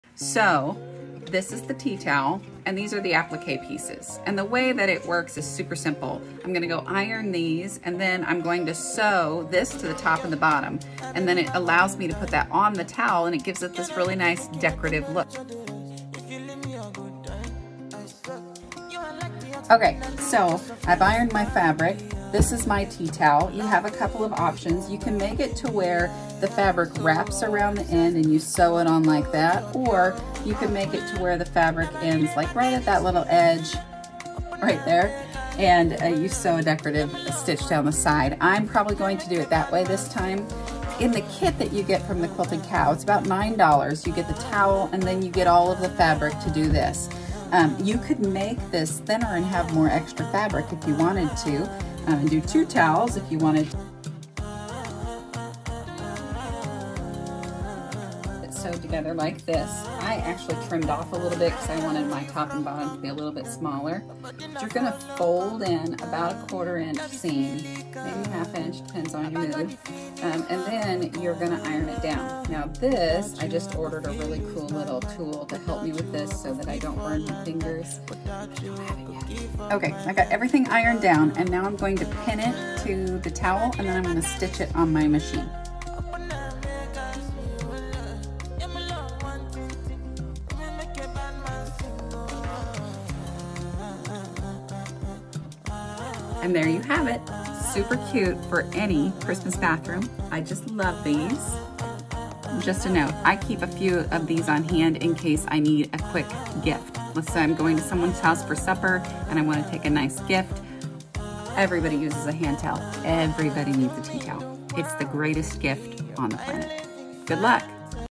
Appliqué Tea Towel
This is a super easy and fun way to dress up a tea towel!  Kits available from the Quilted Cow